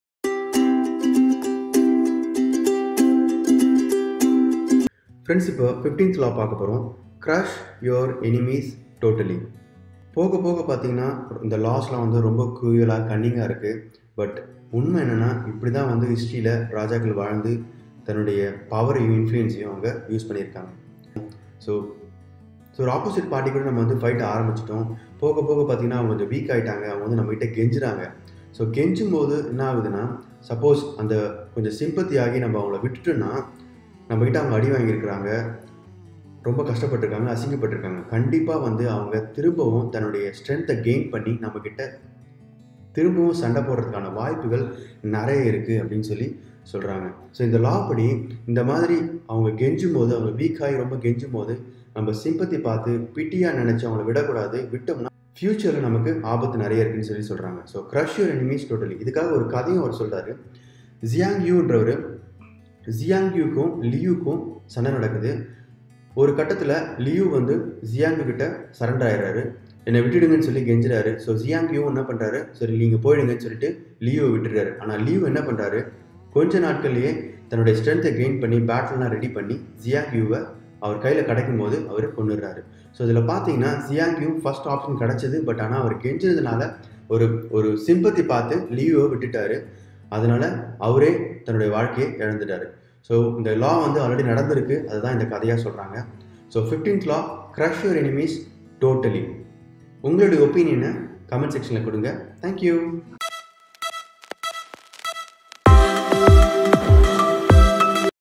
TAMIL 48 LAWS OF POWER #15
to get notifications. TAMIL 48 LAWS OF POWER

The 48 Laws Of Power is by far one of the most powerful books ever written. Each of these laws is used by people in positions of power, or by people who strive for those positions.

The 48 Laws of Power (1998) is a non-fiction book by American author Robert Greene. The book is a bestseller, selling over 1.2 million copies in the United States, and is popular with prison inmates and celebrities.

 CRUSH YOUR ENEMY TOTALLY

Wisdom in a nutshell:
• Show no mercy. Crush your rivals or else you will give them time to regroup and
plot their revenge.
• Banish enemies or plot for the best time to render them harmless.
• Leave your enemies no options.
• Sometimes enemies will destroy themselves.
• Thoughts of reconciliation will open you up to attack.